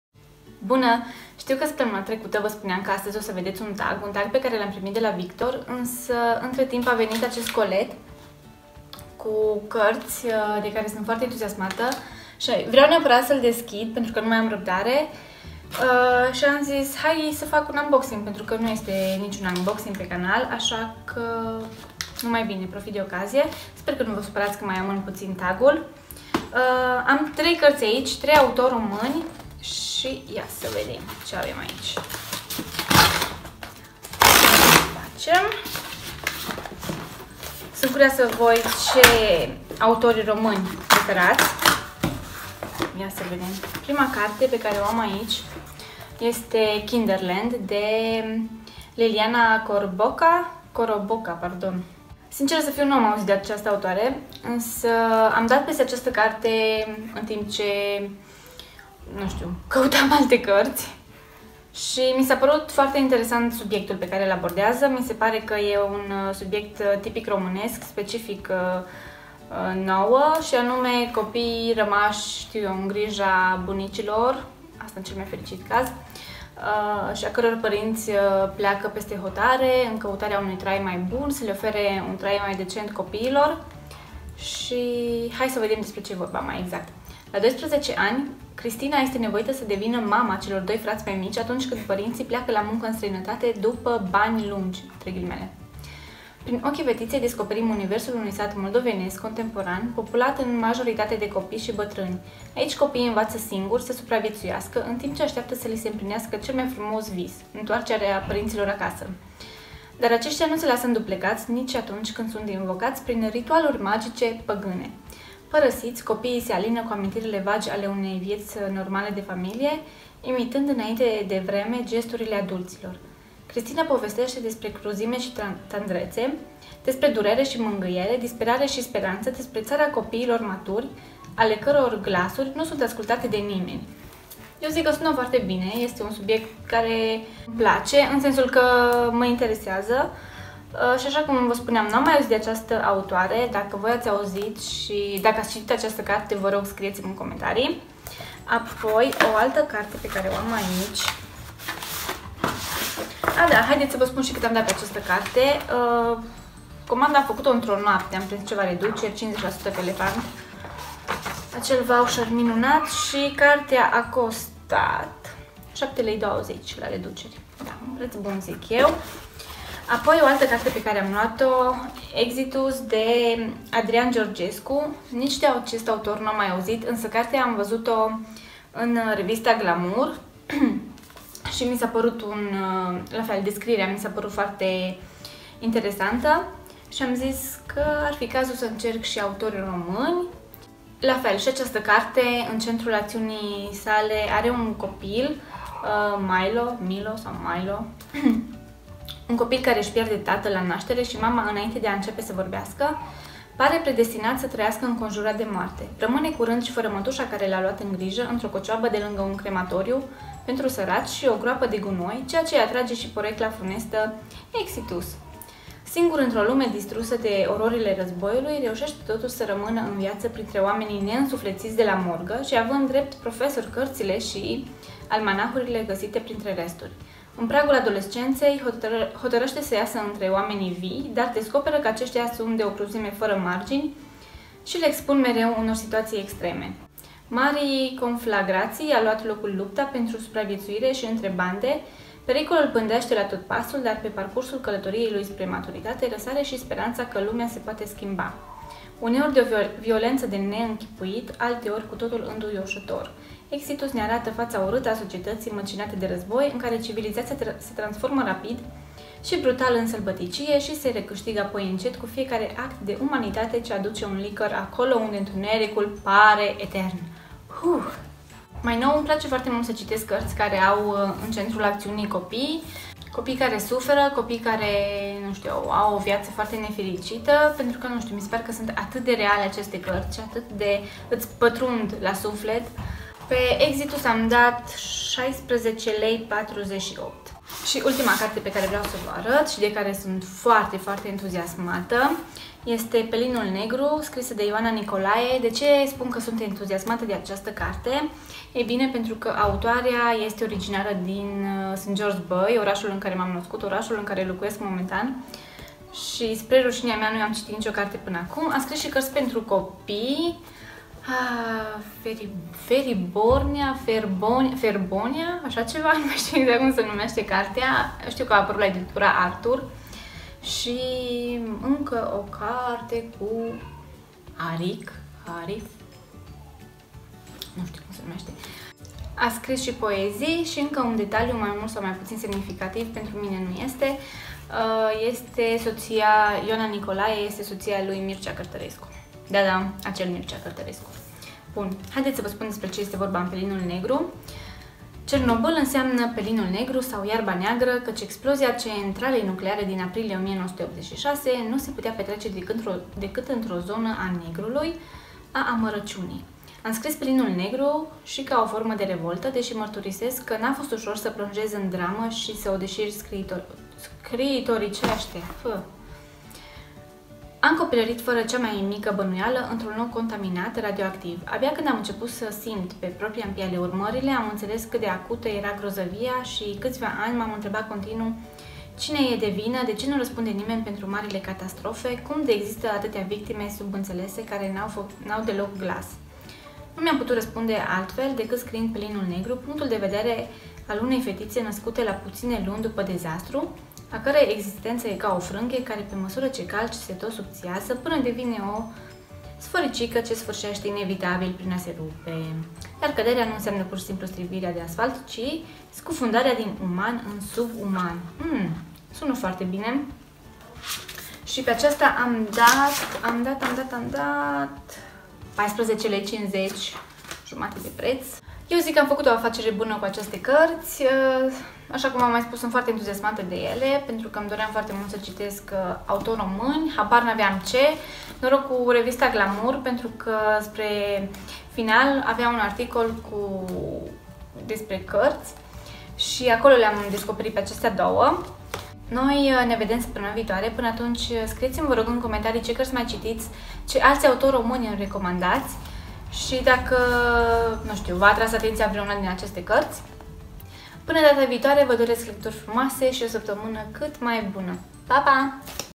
Unboxing Autori Romani
Primul unboxing de pe acest canal... trei carti, trei autori romani :) Nu uitati sa imi spuneti parerea voastra ;)